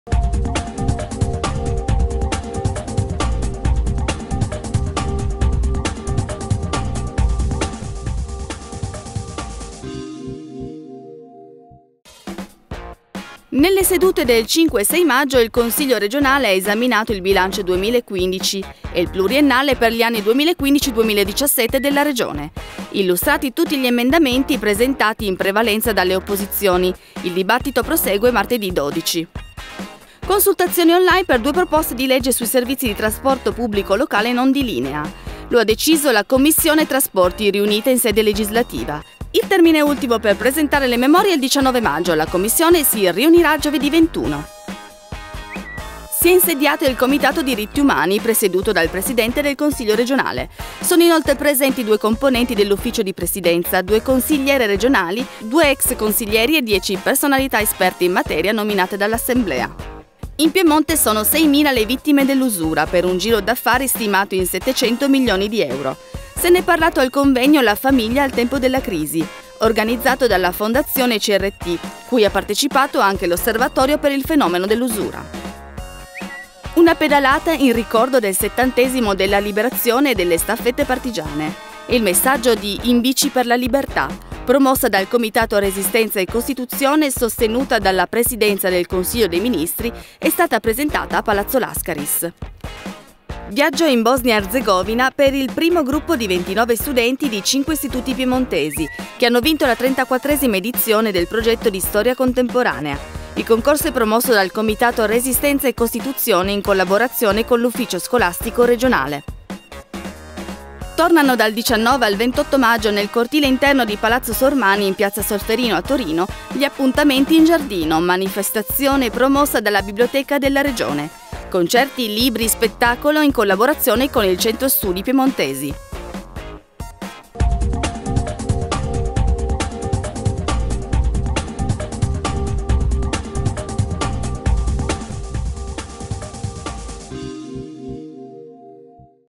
Webtg n. 18 dell’8 maggio 2015
Nelle sedute del 5 e 6 maggio il Consiglio regionale ha esaminato il bilancio 2015 e il pluriennale per gli anni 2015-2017 della Regione. Illustrati tutti gli emendamenti, presentati in prevalenza dalle opposizioni, il dibattito prosegue martedì 12.

Consultazioni online per due proposte di legge sui servizi di trasporto pubblico locale non di linea. Lo ha deciso la Commissione Trasporti, riunita in sede legislativa. Il termine ultimo per presentare le memorie è il 19 maggio, la Commissione si riunirà giovedì 21.

Si è insediato il Comitato Diritti Umani, presieduto dal presidente del Consiglio regionale. Sono inoltre presenti due componenti dell'Ufficio di presidenza, due consigliere regionali, due ex consiglieri e dieci personalità esperte in materia nominate dall’Assemblea.

In Piemonte sono 6.000 le vittime dell'usura, per un giro d’affari stimato in 700 milioni di euro. Se ne è parlato al convegno “La famiglia al tempo della crisi”, organizzato dalla Fondazione Crt, cui ha partecipato anche l’Osservatorio per il fenomeno dell’usura.

Una pedalata in ricordo del 70° della Liberazione e delle staffette partigiane. È il messaggio di “In bici per la libertà”. Promossa dal Comitato Resistenza e Costituzione, sostenuta dalla presidenza del Consiglio dei Ministri, è stata presentata a Palazzo Lascaris.

È partito per la Bosnia Erzegovina il primo gruppo di 29 studenti di 5 istituti piemontesi che hanno vinto la 34a edizione del progetto di storia contemporanea. Il concorso è promosso dal Comitato Resistenza e Costituzione in collaborazione con l’Ufficio scolastico regionale.

Tornano, dal 19 al 28 maggio, nel cortile interno di Palazzo Soriani, in piazza Solferino a Torino, gli appuntamenti in giardino, manifestazione promossa dalla Biblioteca della Regione. Concerti, libri, spettacolo, in collaborazione con il Centro Studi piemontesi.